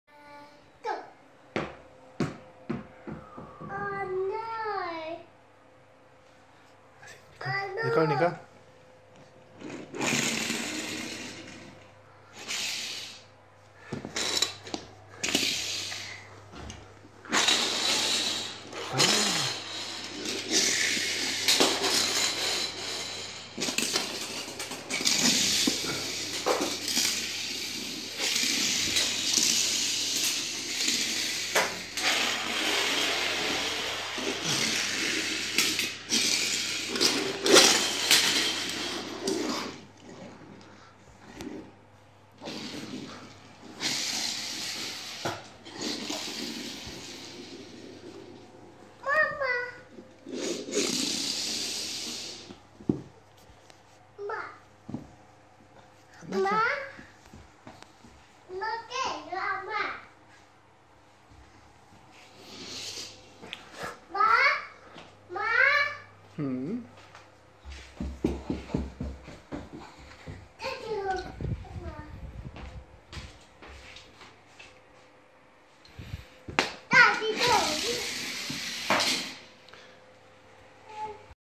Little Disney Cars Toys Race with a Female Toddler
Little Disney Cars Toys race with a female toddler in London, UK